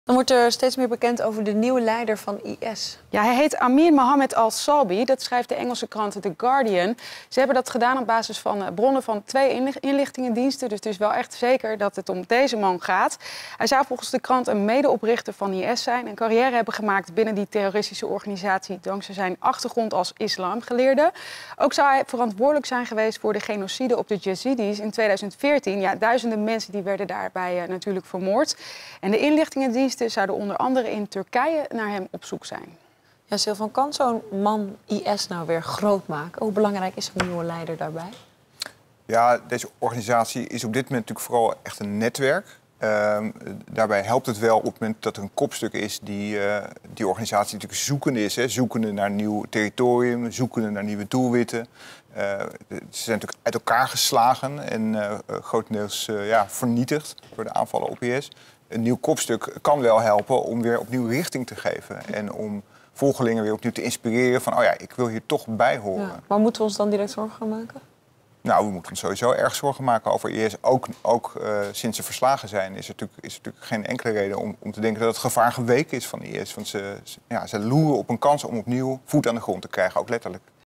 Dit is de nieuwe leider van Islamitische Staat: 'Gevaar is niet geweken'
Er wordt steeds meer bekend over de nieuwe leider van Islamitische Staat. The Guardian meldt dat Amir Mohammed Abdul Rahman al-Mawli al-Salbi de gedoodverfde opvolger van de omgekomen Abu Bakr al-Baghdadi is. De Britse krant meldt dat op basis van bronnen bij twee verschillende inlichtingendiensten.